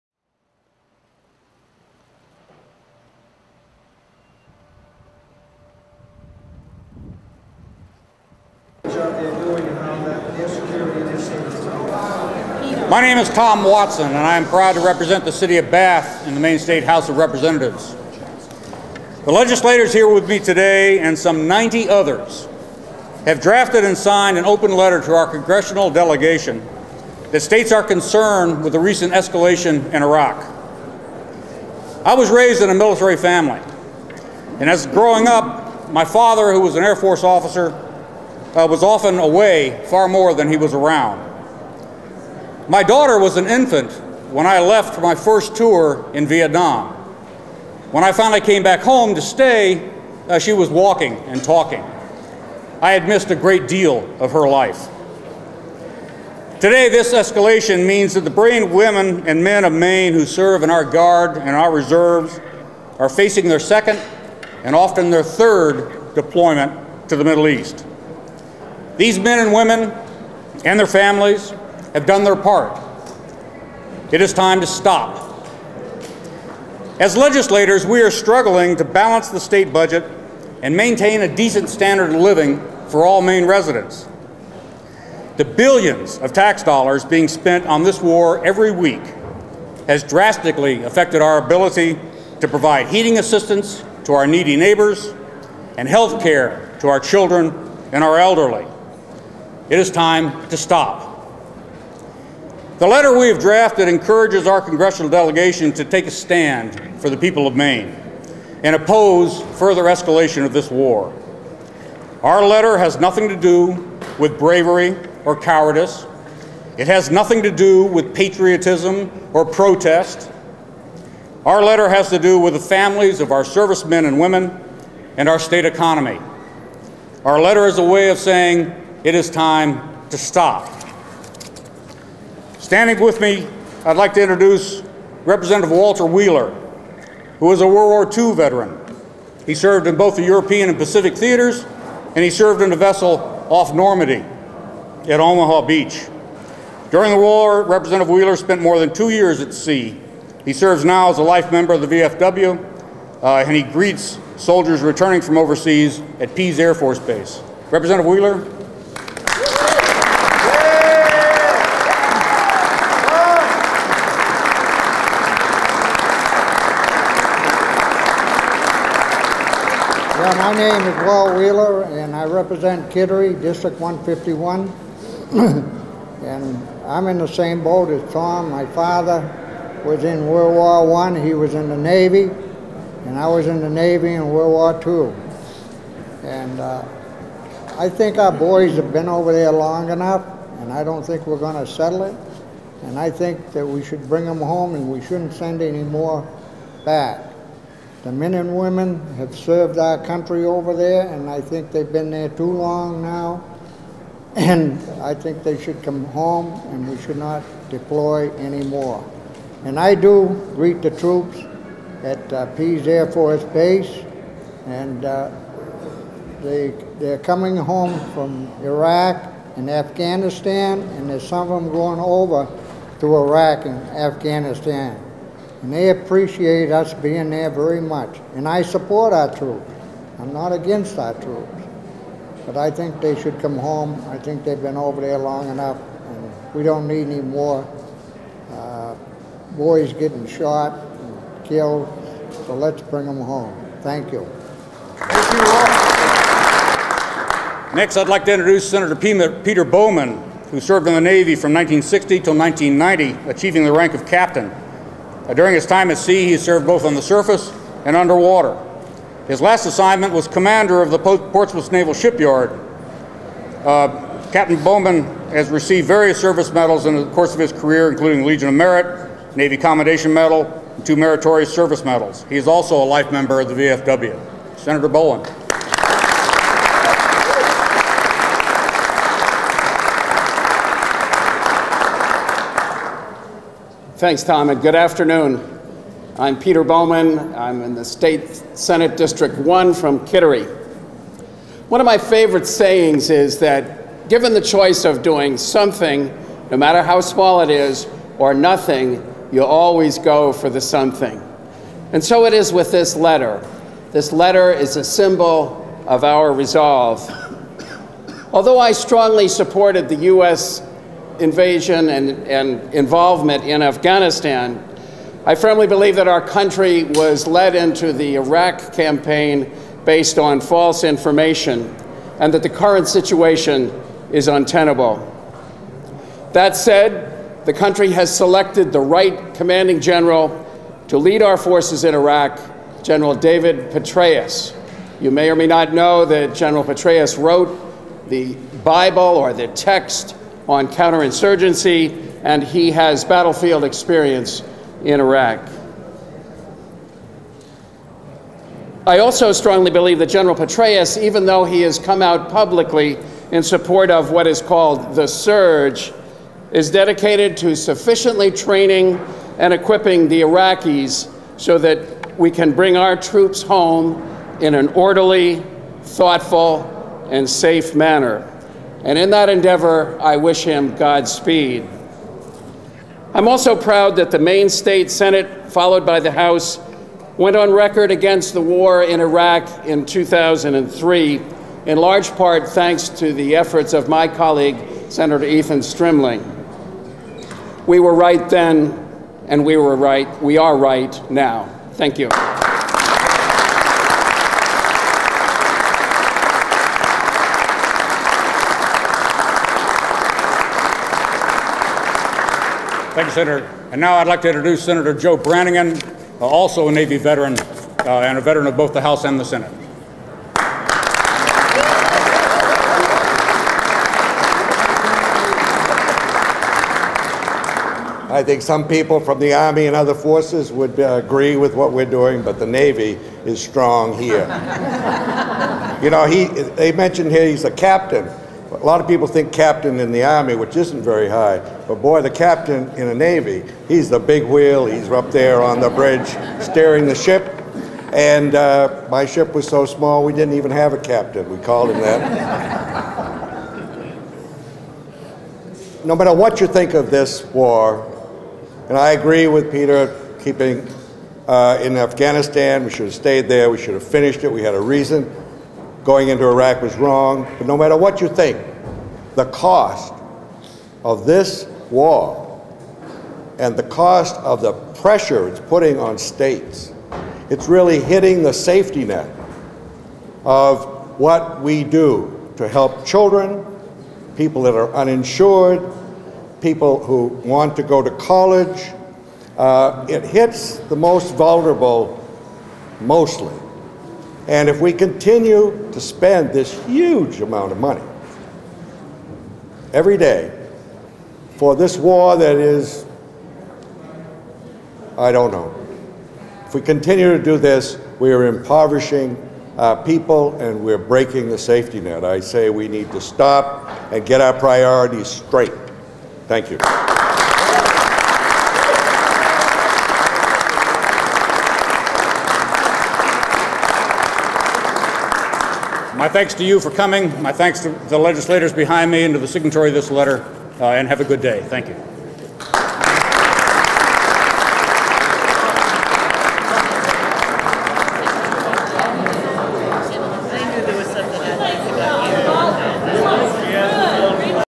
Maine Legislators oppose surge,escalation
State lawmakers from the House and Senate submitted a letter urging congress to oppose Iraq troop escalation.  Their press conference was held at the state house Thursday Feb 15